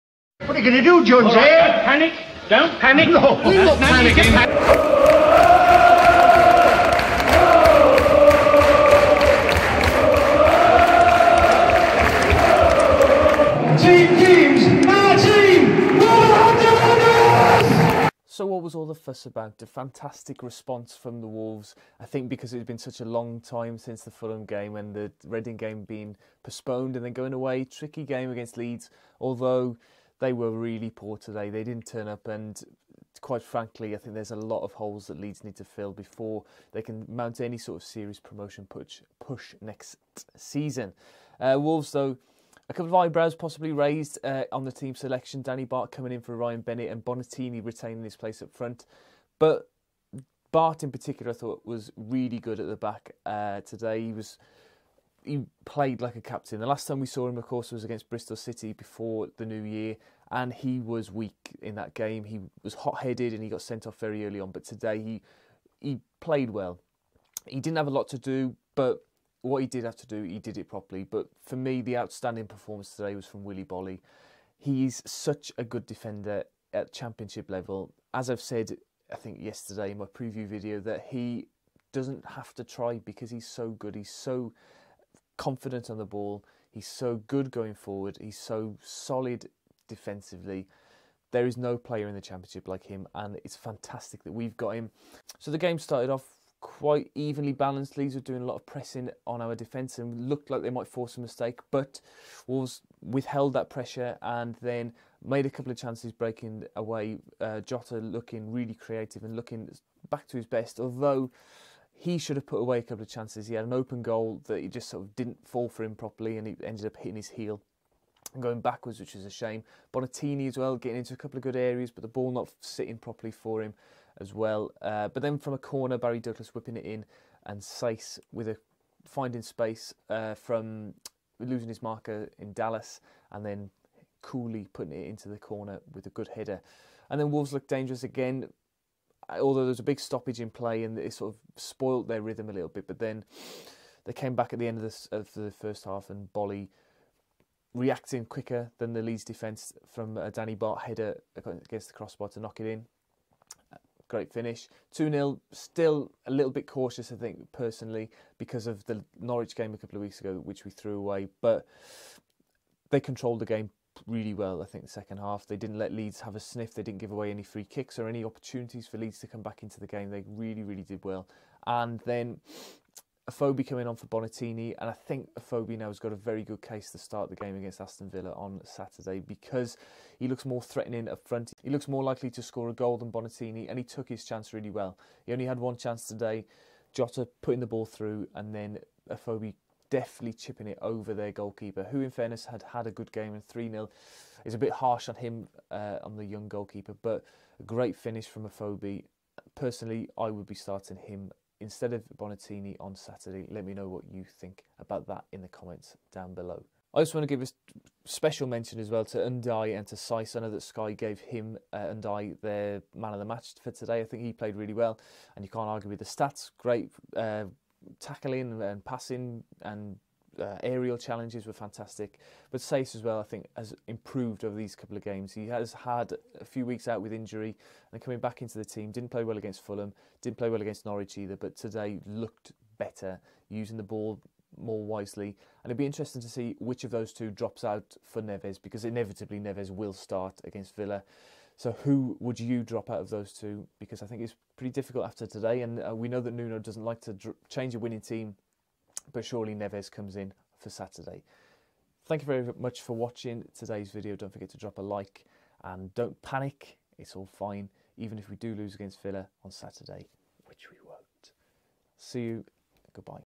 Leeds United 0-3 Wolves | Match Review
Goals from Saiss, Boly and Afobe provided a fantastic response today. Chance to extend the gap now on Saturday.